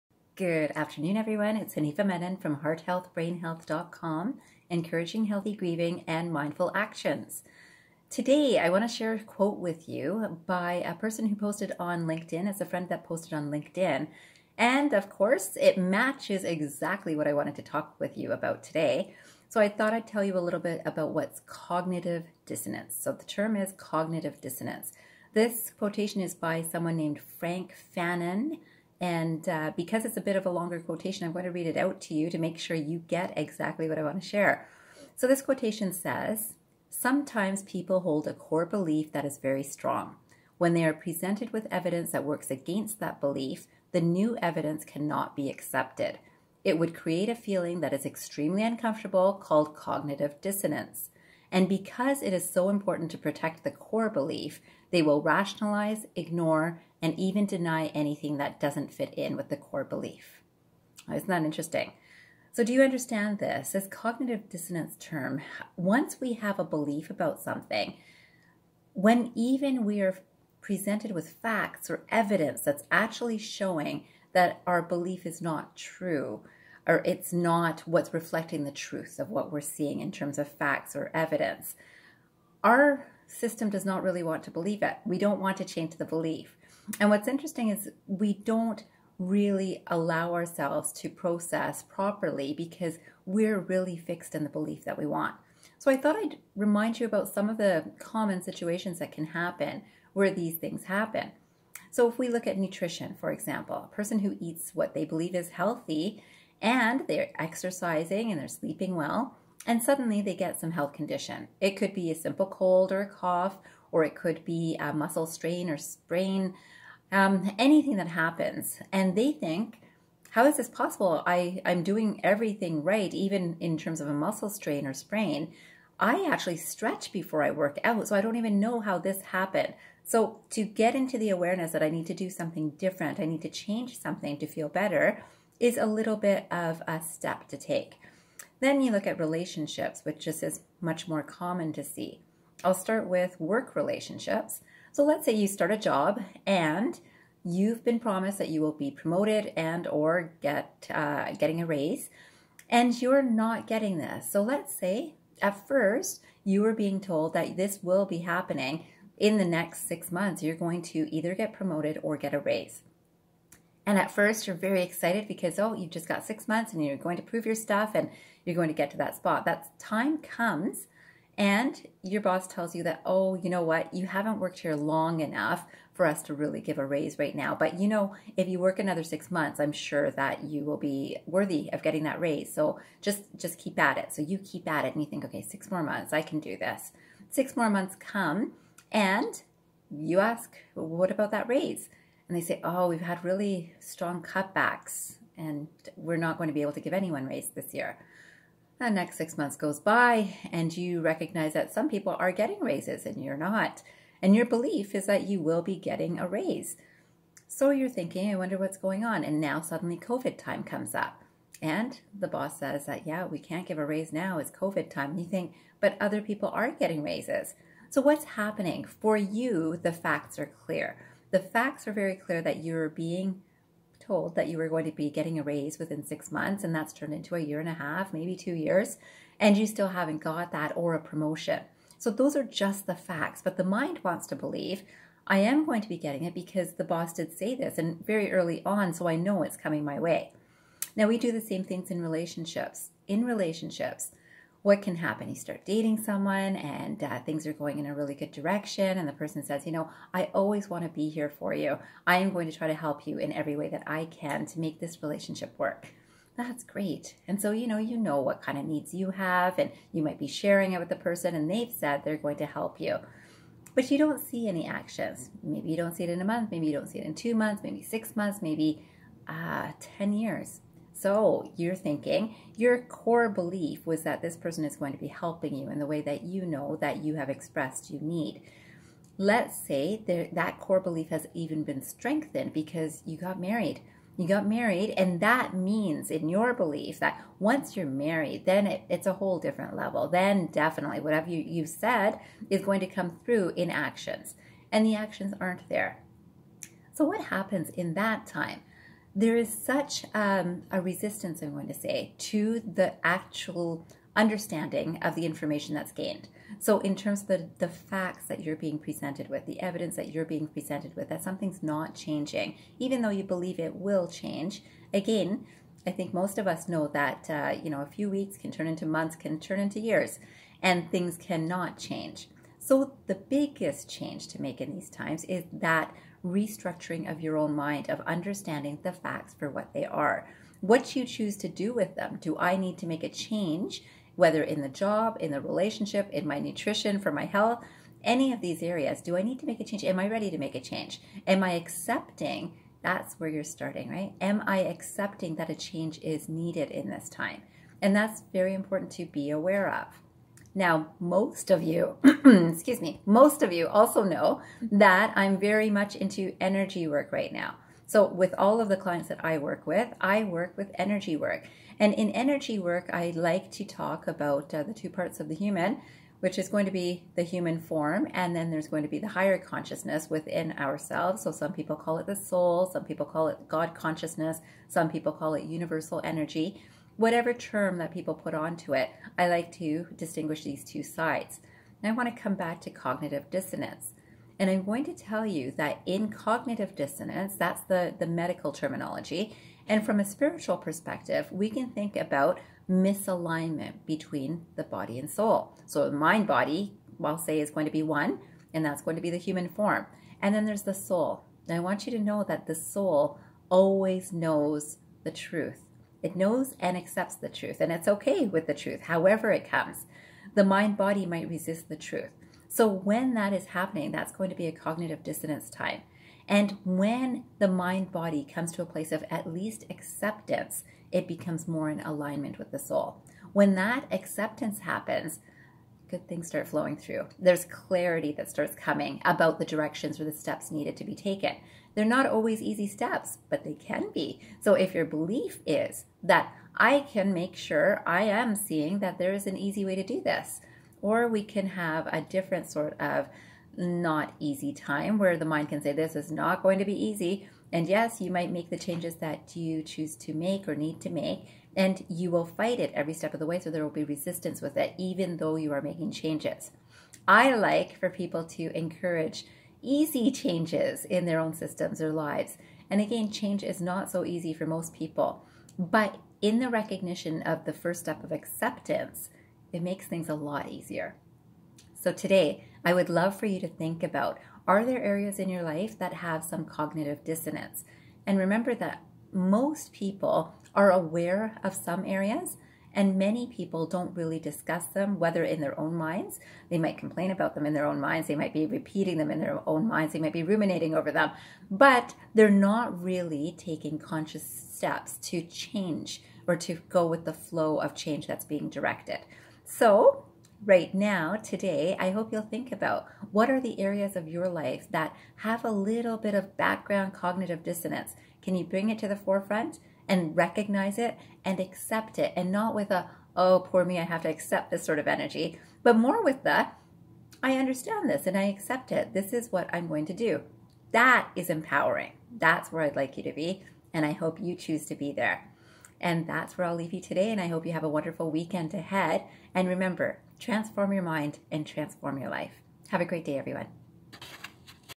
Cognitive Dissonance and Spirituality
Have you heard about the term "cognitive dissonance"? Many who have some Psychology training or experience may be familiar with this. Relating it to Spirituality can also help you know what the discomfort is all about!